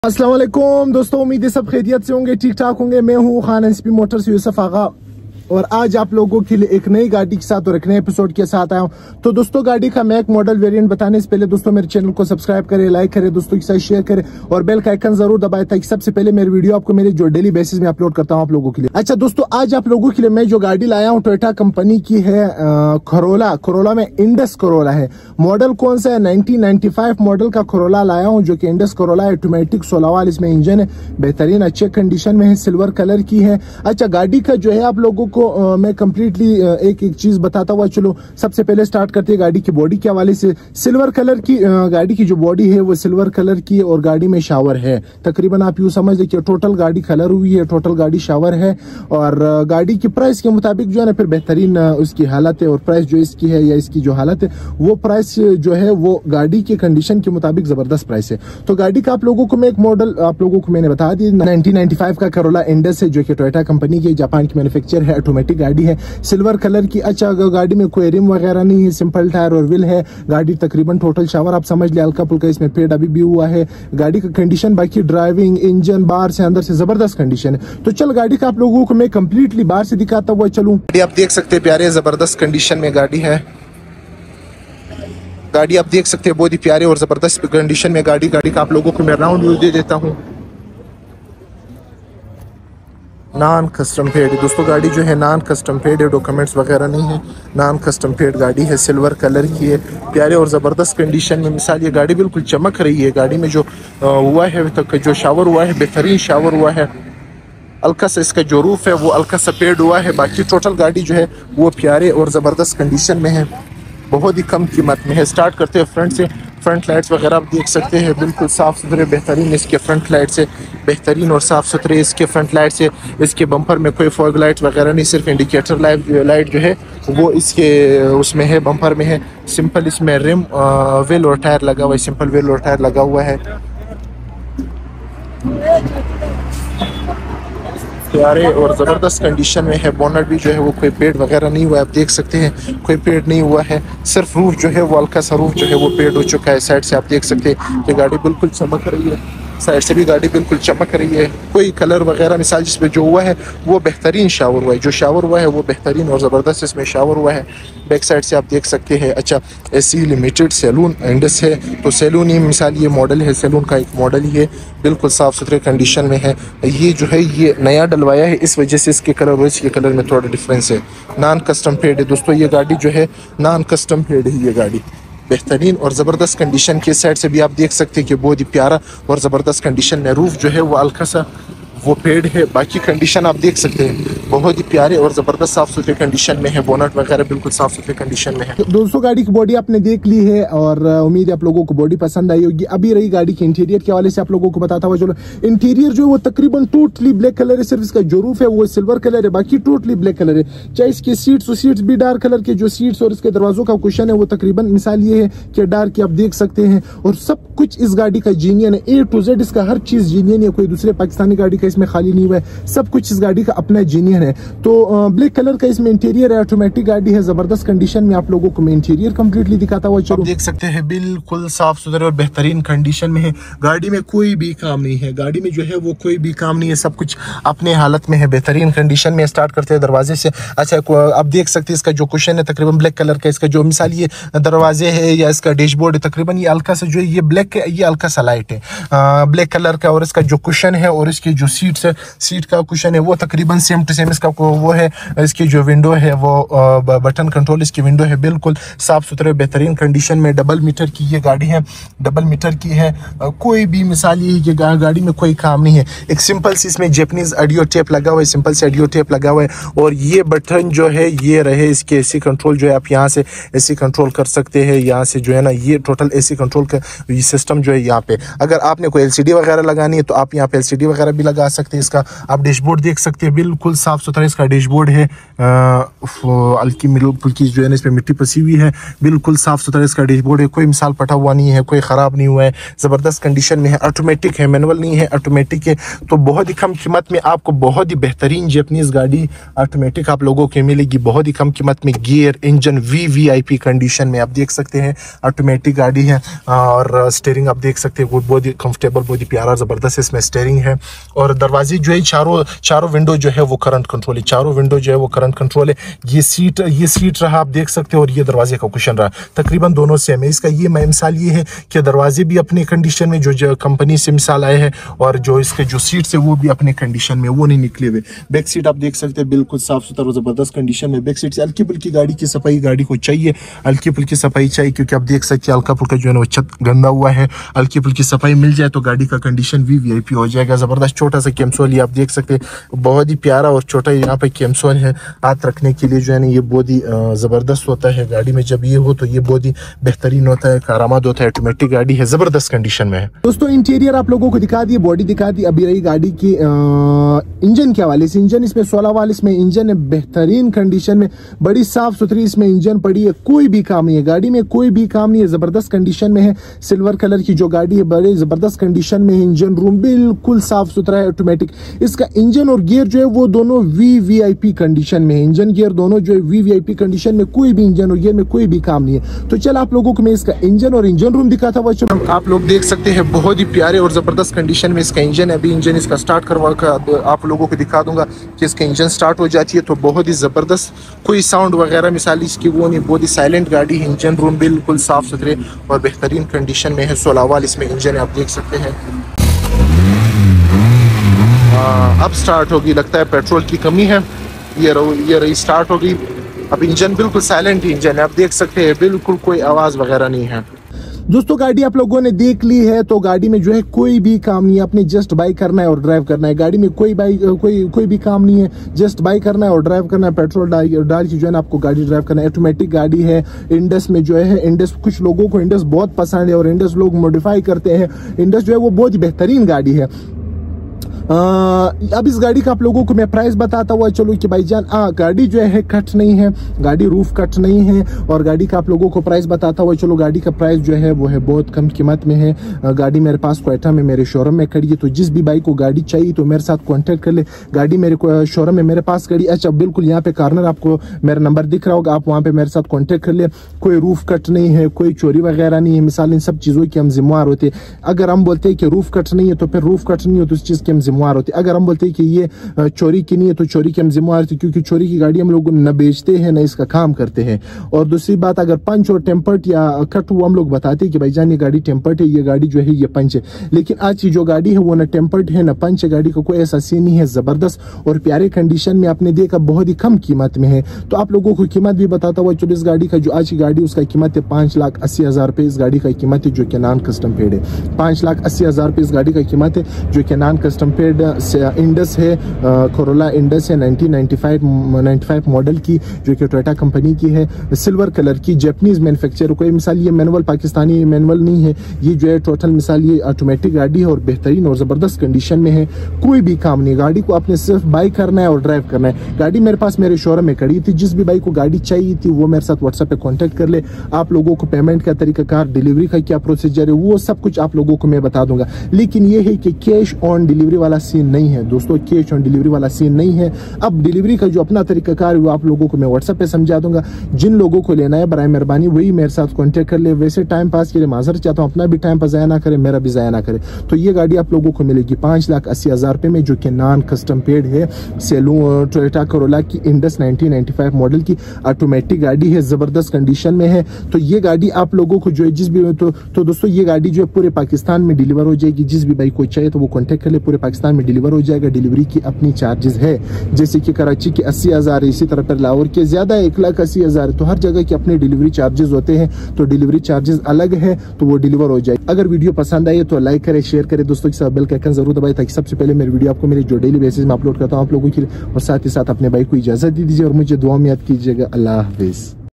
TOYOTA SALOON INDUS 16 valve automatic (1995) NCP | non custom paid | NCP cars in Quetta balochistan
Present car is
Toyota
Saloon
Indus
Model 1995
Engine 16 valve
Color silver
Automatic
Location Quetta
Price 5.80

Every type of non custom paid cars are available

Delivery available to all Pakistan with extra charges